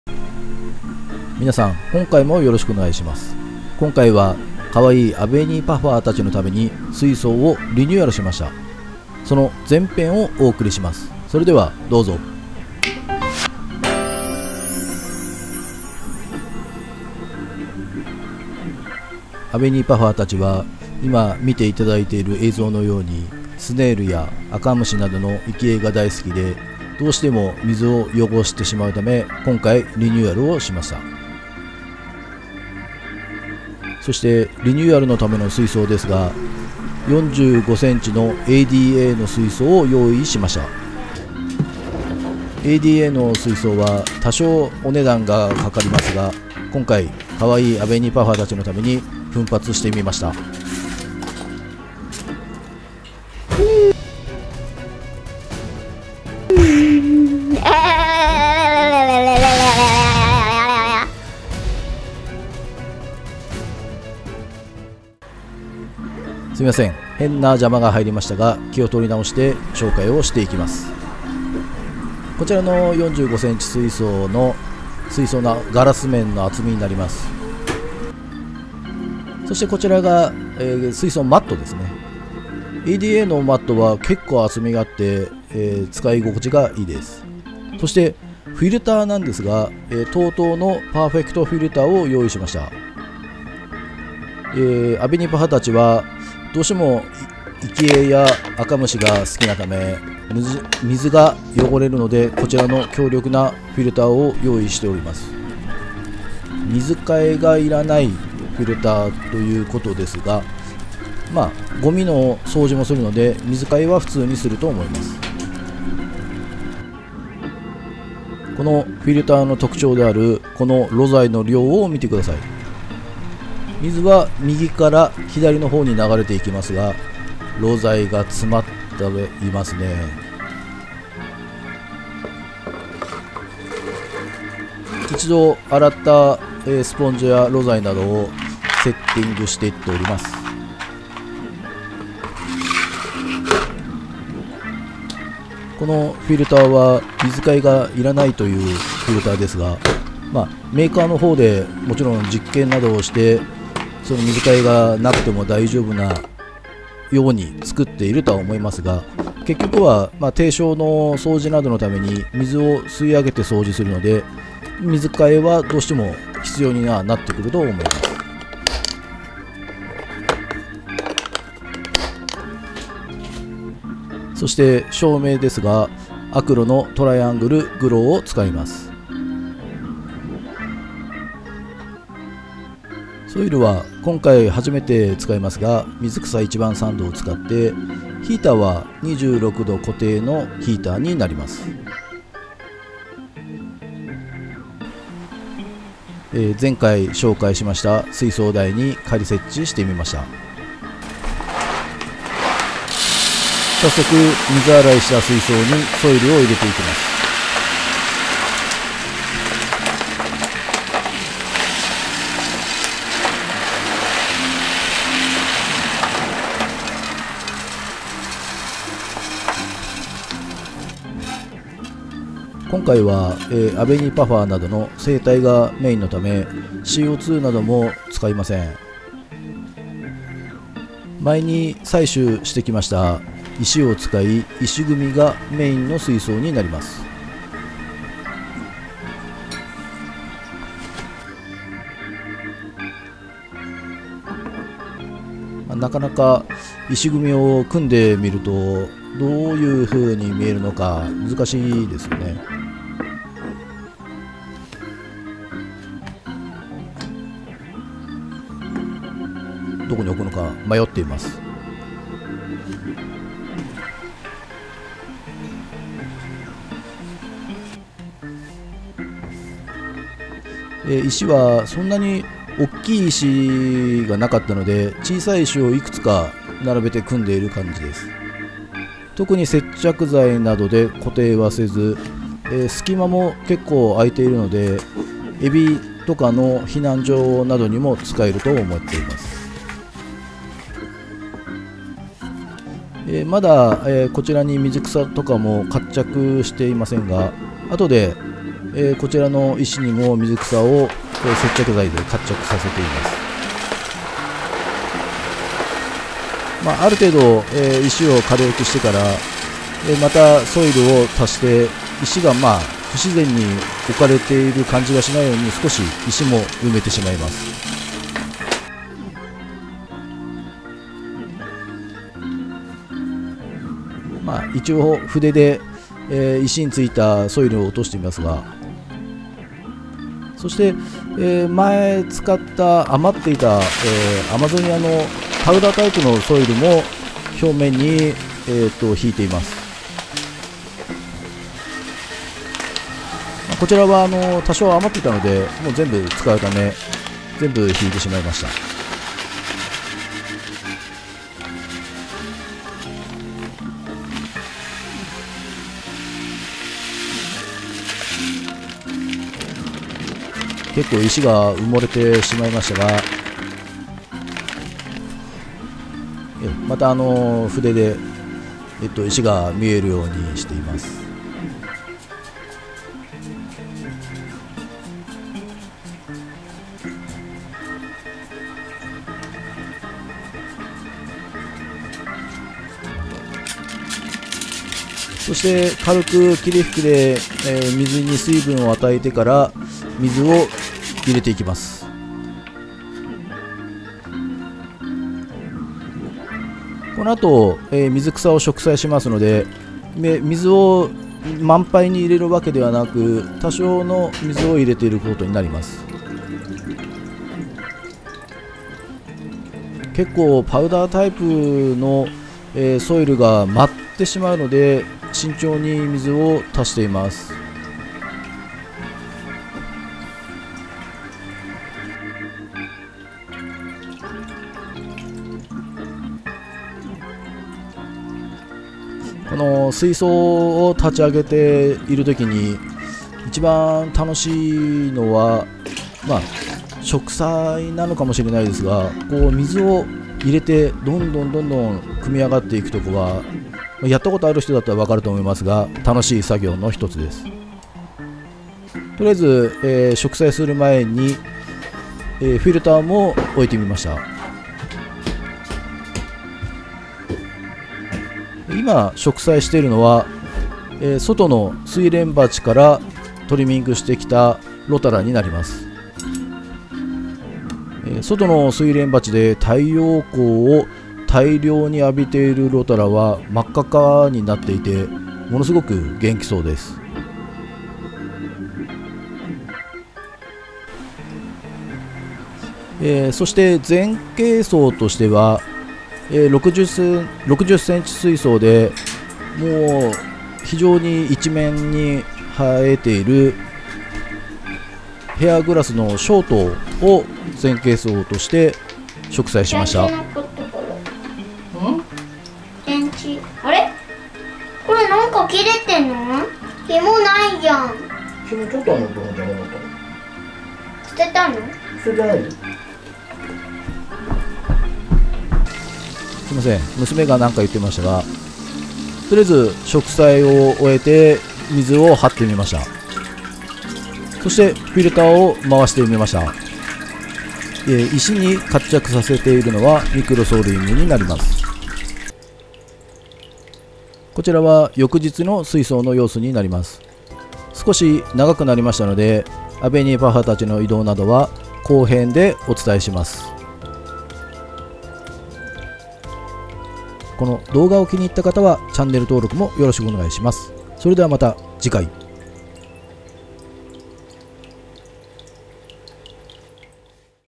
アベニーパファー水槽立ち上げリニューアル（前編）
カワイイ淡水フグのアベニーパファー水槽をリニューアル！
貝やアカムシが好きなアベニーパファー達は水を汚すので
前よりも少し大きな水槽で、強力なフィルターをセットしました。